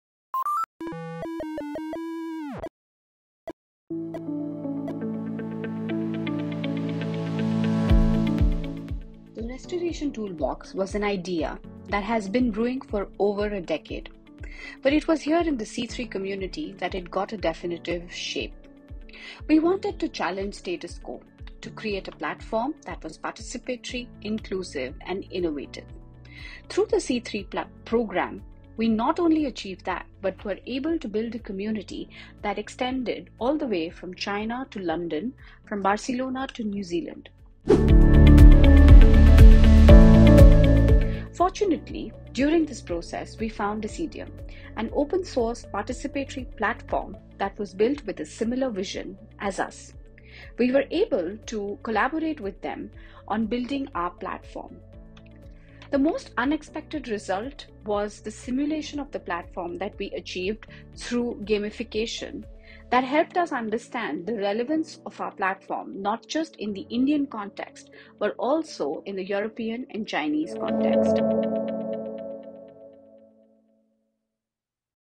Restoration Toolbox
Restoration Toolbox: An open source platform to enable participation from various communities to preserve their heritage in a secure way.

Lead Investigator: Aishwarya Tipnis
Group Members: Hinna Devi Singh, Manan Goyal, Marsha Bradfield

The Restoration Toolbox is an open sourced digital platform targeted towards empowering local communities, city authorities, NGOs, experts, and practitioners to come together to preserve local heritage.

Overall, the toolbox is meant to return agency to communities and stakeholders to take action in preserving their heritage.